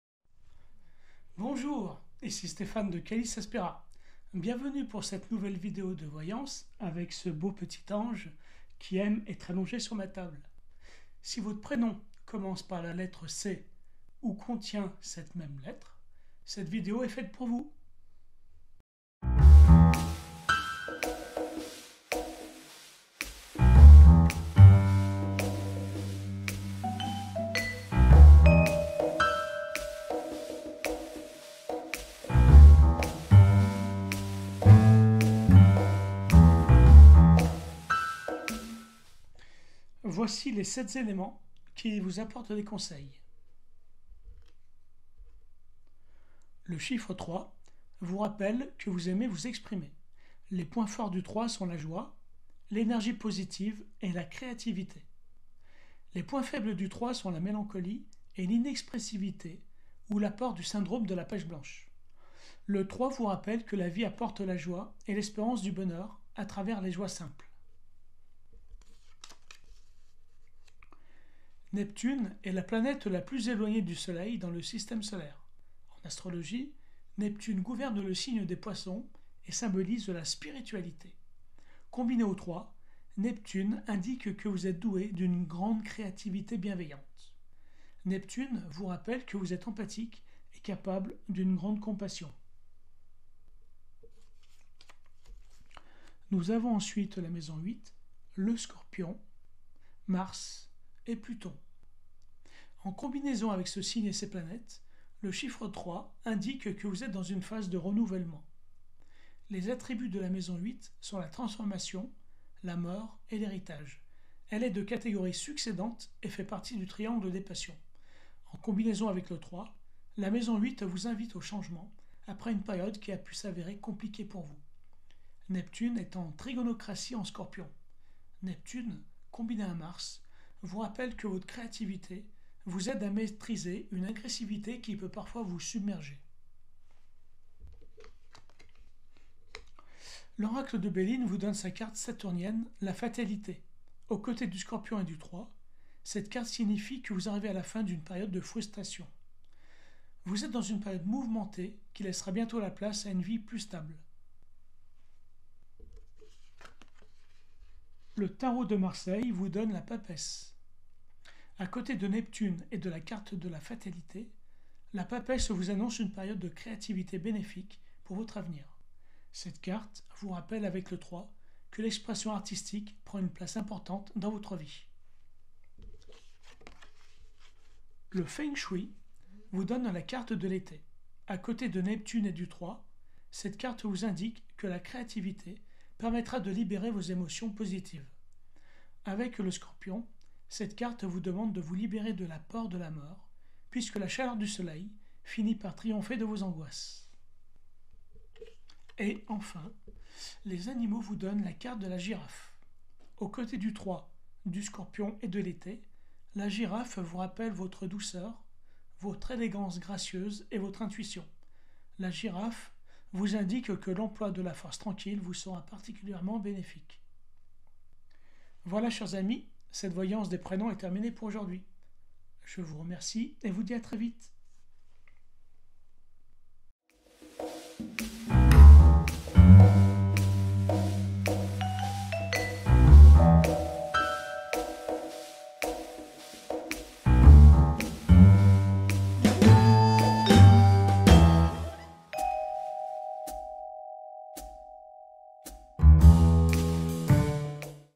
La voyance des prénoms - Lettre C
Bonjour,

Voici une voyance des prénoms. Aujourd'hui cette vidéo est destinée à ceux dont le prénom commence par la lettre C ou contient la lettre C.

Stéphane

▬▬▬▬▬▬▬ LES CHAPITRES ▬▬▬▬▬▬▬
00:00 Introduction
00:18 Générique de début
00:39 Le Chiffre
01:09 La Planète
01:33 La Maison Astrologique
02:17 Oracle de Belline
02:36 Tarot de Marseille
02:56 Feng-Shui
03:18 L'Animal Totem
03:46 Générique de fin

▬▬▬▬▬▬▬ REALISATION, TOURNAGE, MONTAGE ▬▬▬▬▬▬▬
Callis Aspera

▬▬▬▬▬▬▬ CREDITS ▬▬▬▬▬▬▬
Générique